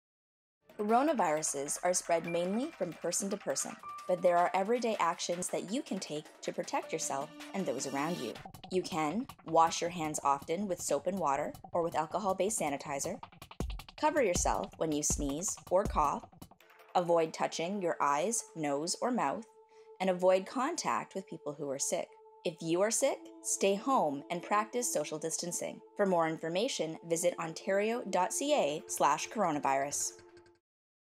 COVID-19 Steps to Take to Stay Healthy
What you need to know to help you and your family stay healthy through COVID-19.